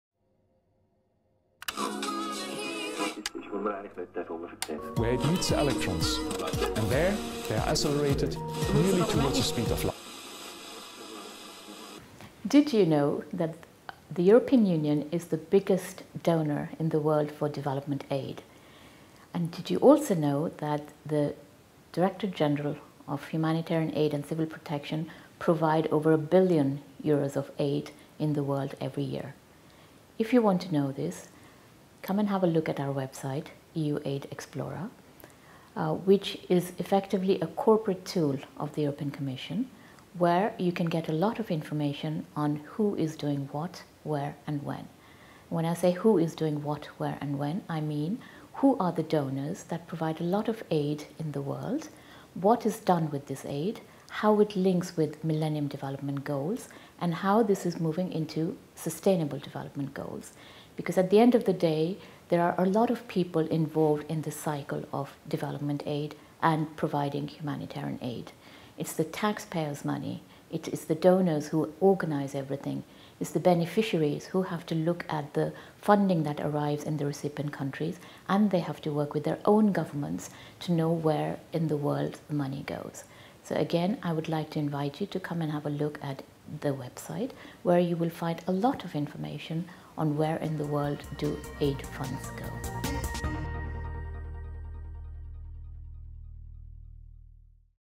Anjula Garg, JRC Scientist - Development Aid
The Joint Research Centre, the European Commission's in-house science service, presents its Faces of Science series - showcasing the human face of the scientific advice supporting EU policy.